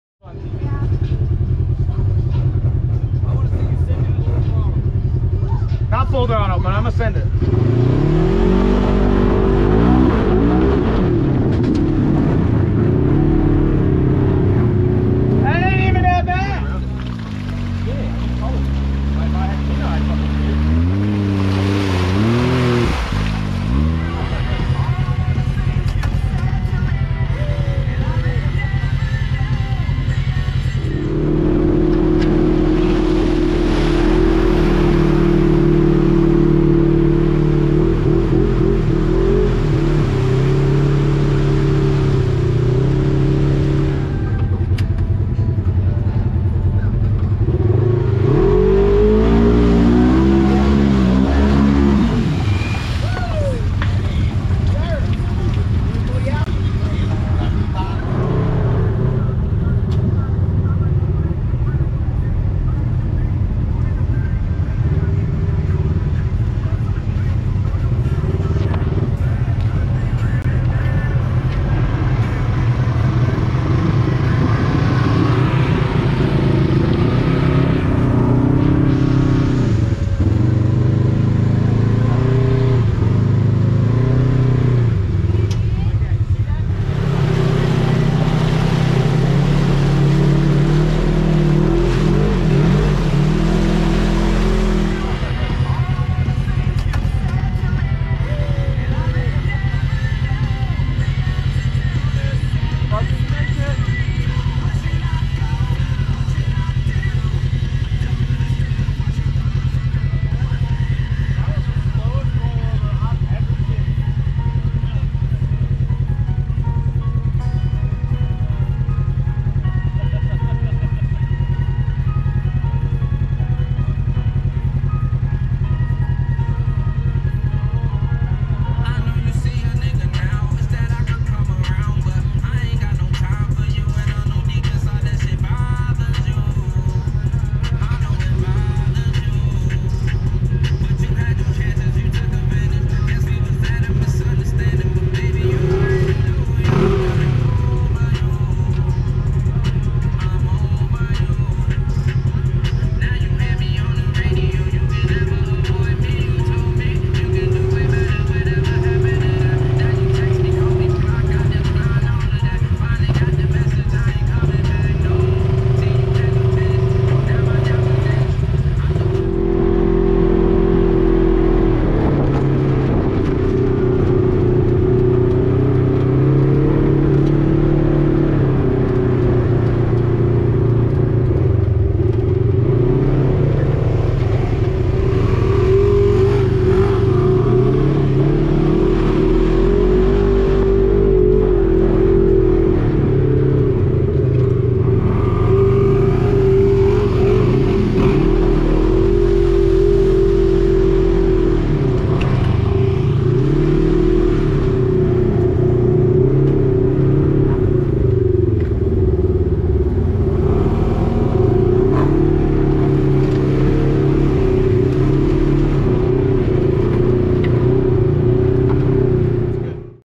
Exploring Moccasin Creek Off Road Park
Went on a Day trip with the Squad! had a blast, shoulda got more video but was to busy riding! Enjoy!